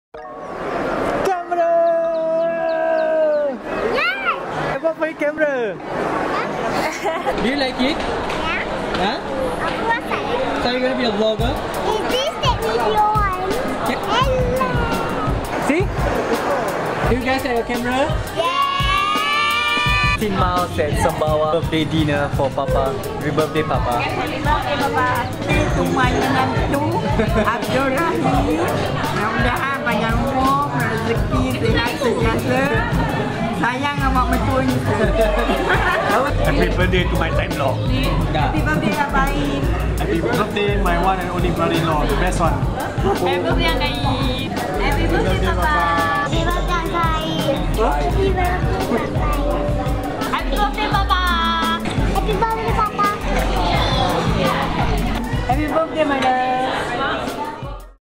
The kids camera cost 30 dollars!
UHD video.
5megapixel.

Something for the kids to enjoy and have fun, rather than just playing their handphone!

Vlogging camera: xt3
Microphone: Rode mic go